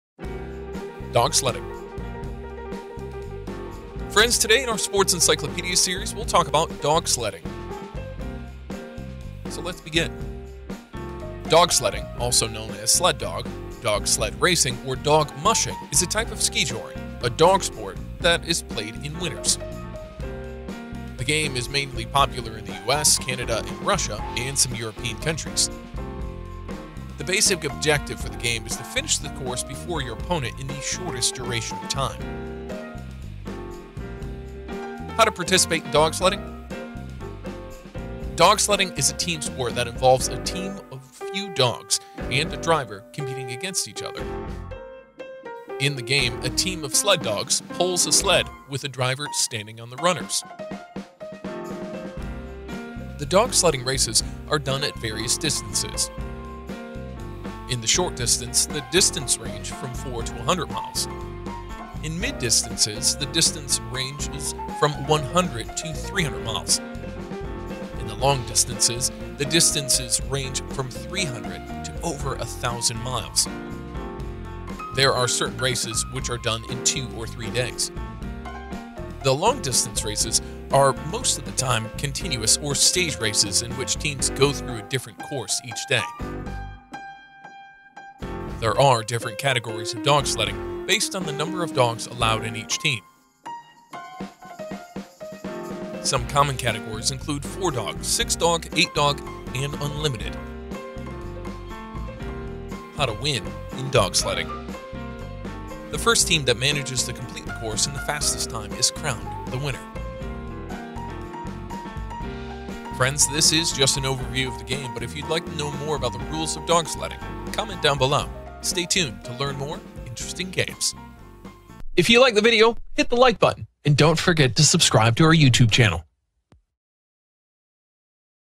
How is Dog Sledding Played? (also known as a dog sled racing, or dog mushing is a type of Skijoring)
Timestamps OR Chapters:
0:00 What is Dog Sledding?
0:34 How To Play Dog Sledding?
1:42 How to Win in Dog Sledding?

Friends, today in our sports encyclopedia series we will talk about Dog Sledding.
Dog Sledding, also known as a sled dog, dog sled racing, or dog mushing is a type of Skijoring, a dog sport that is played in winters.
The game is mainly popular in the USA, Canada, Russia, and some European countries.
The basic objective for the game is to finish the course before your opponent in the shortest duration of time.
Dog sledding is a team sport that involves a team of few dogs and a driver competing against each other.
In the game, a team of sled dogs pulls a sled with the driver standing in the runners.
The dog sledding races are done at various distances.
In the short distance, the distance ranges from 4 to 100 miles.
In the mid-distances, the distance ranges from 100 to 300 miles.
In the long distances, the distance ranges from 300 to over 100 miles.
There are certain races which are done in two or three days
The long-distance races are most of the time-continuous or stage races in which teams go through a different course every day.
There are different categories of dog sledding based on the number of dogs allowed in each team.
Some common categories include four-dog, six-dog, eight-dog, ten-dog, and unlimited.
The first team that manages to complete the course in the fastest time is crowned the winner.